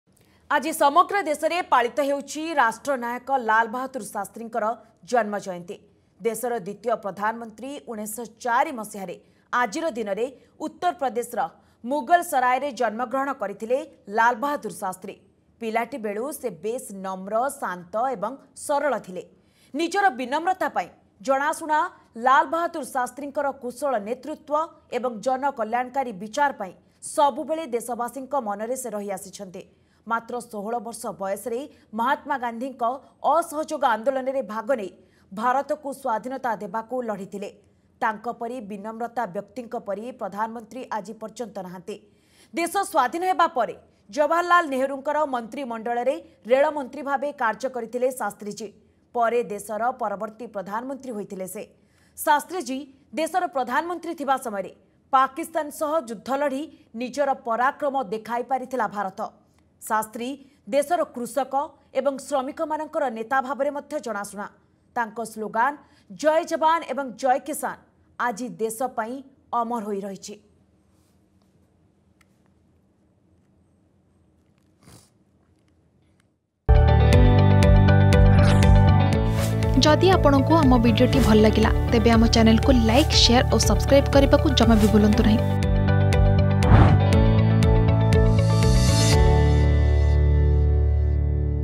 ରାଷ୍ଟ୍ରନାୟକ ଲାଲ ବାହାଦୂର ଶାସ୍ତ୍ରୀଙ୍କ ଜନ୍ମ ଜୟନ୍ତୀ | Lal Bahadur Shastri birthAnniversary | Argus News
ଆଜି ସମଗ୍ର ଦେଶରେ ପାଳିତ ହେଉଛି ରାଷ୍ଟ୍ରନାୟକ ଲାଲ ବାହାଦୂର ଶାସ୍ତ୍ରୀଙ୍କ ଜନ୍ମ ଜୟନ୍ତୀ । ଦେଶର ଦ୍ବିତୀୟ ପ୍ରଧାନମନ୍ତ୍ରୀ ୧୯୦୪ ମସିହାରେ ଆଜିର ଦିନରେ ଉତ୍ତରପ୍ରଦେଶର ମୁଗଲସରାୟରେ ଜନ୍ମ ଗ୍ରହଣ କରିଥିଲେ ଲାଲ ବାହାଦୂର ଶାସ୍ତ୍ରୀ।